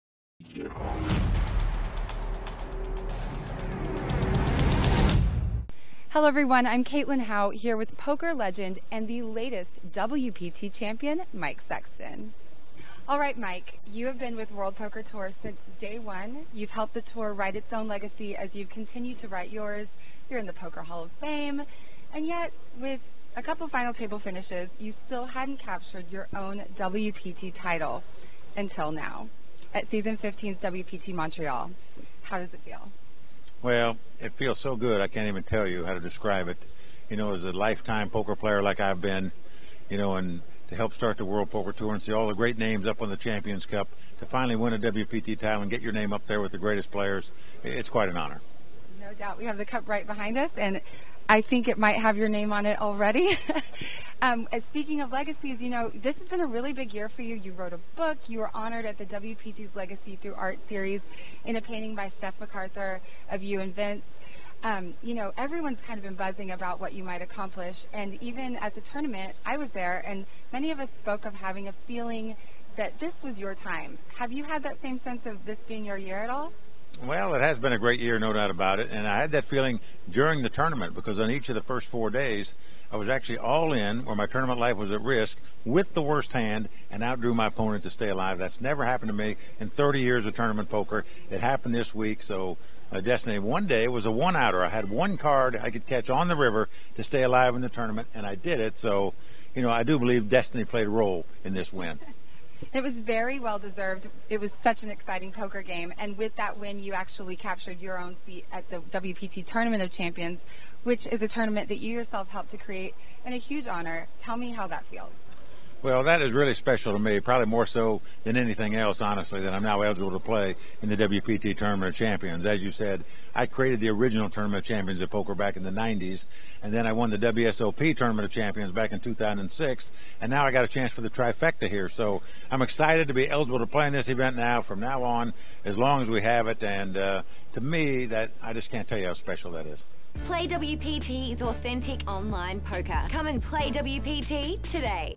Mike Sexton - Poker's Ambassador (part one of three)
In this first installment of a three-part series, Caitlyn Howe interviews Mike Sexton, as he reminisces about his tournament win of the partypoker.net WPT Montreal main event.

Sick of the all-in fests at those other social poker sites?